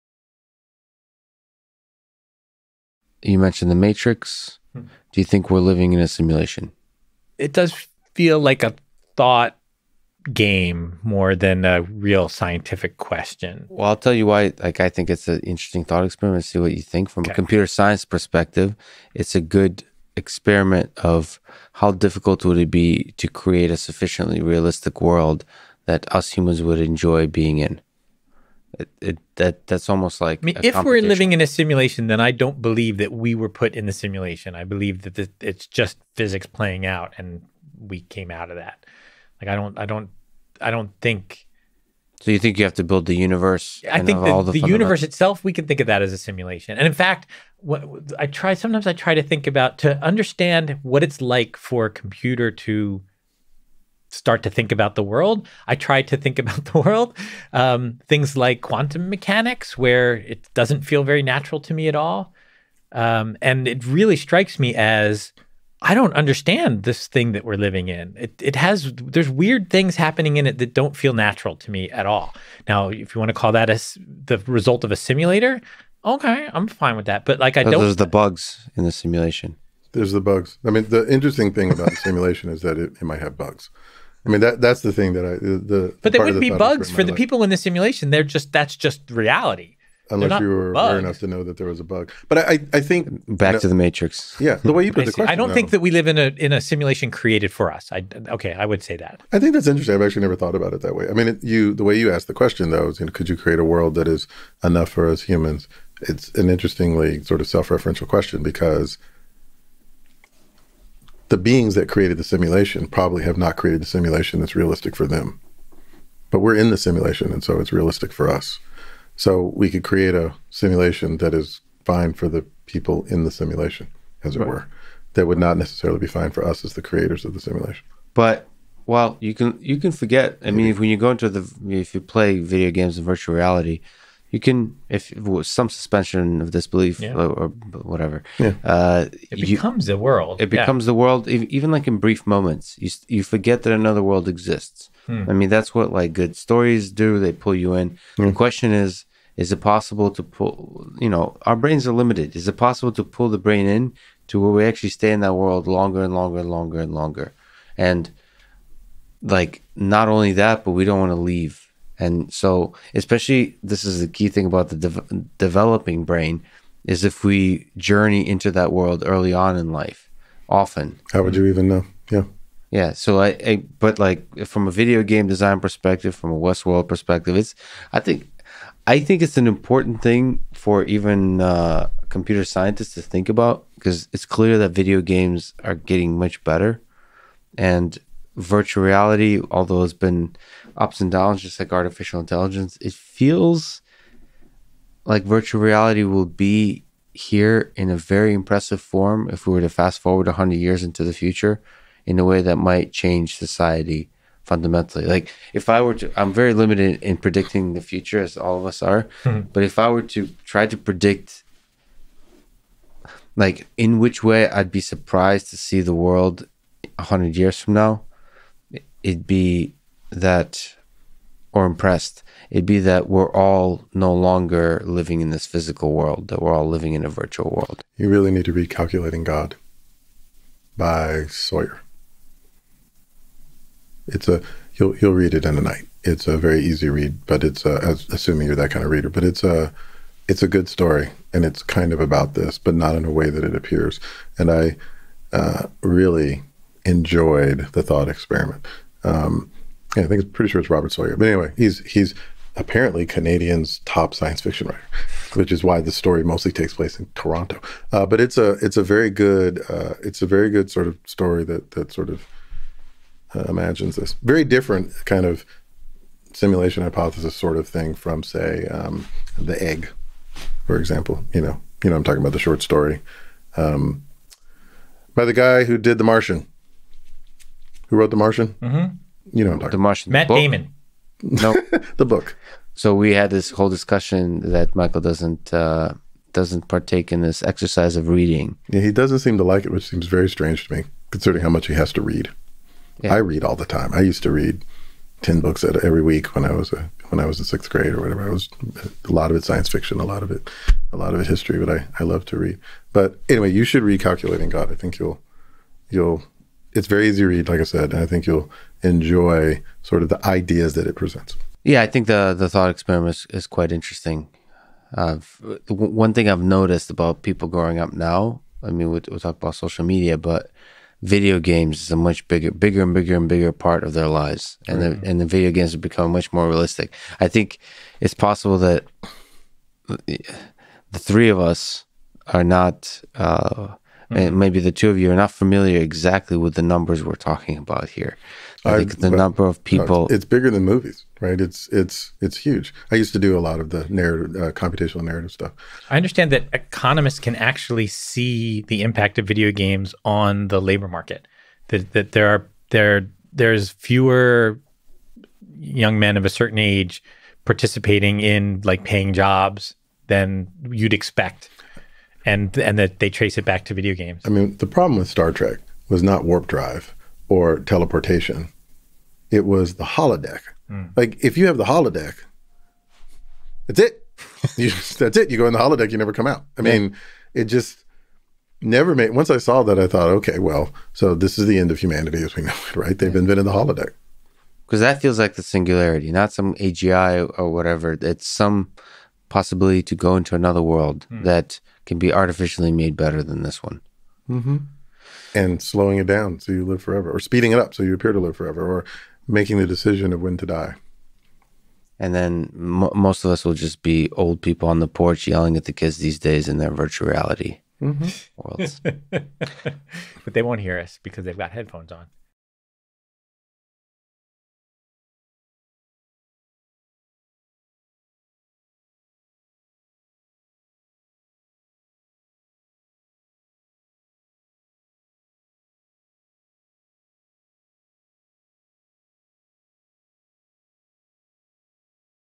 The simulation was not created for us | Charles Isbell and Michael Littman and Lex Fridman
- MasterClass to get 2 for price of 1